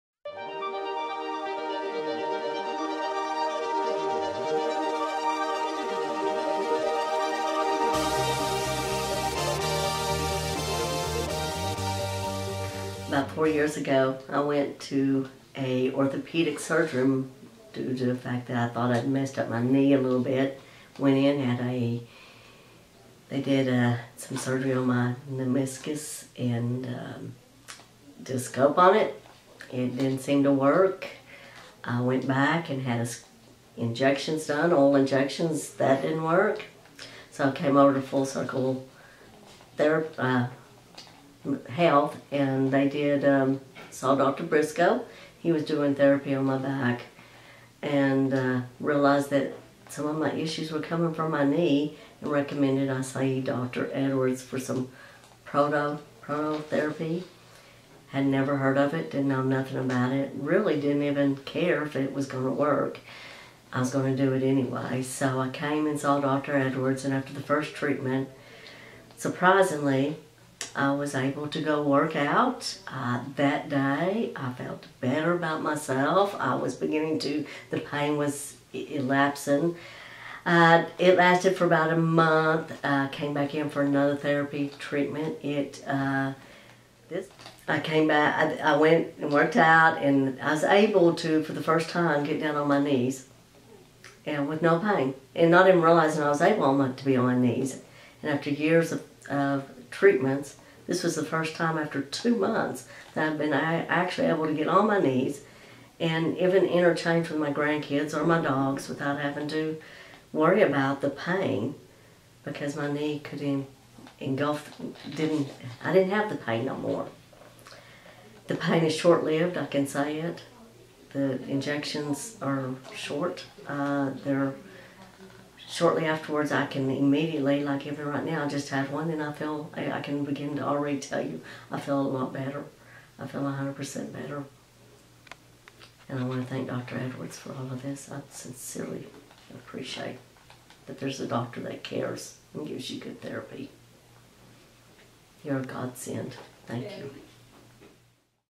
Knee Prolotherapy at Revolution Health & Wellness Clinic
Debbie received prolotherapy for her chronic knee pain with amazing results. She had knee surgery which actually made her pain worse! She received multiple different types of injections and nothing helped until prolotherapy.
She is nearly pain-free after 2 rounds of prolotherapy! Watch her story here.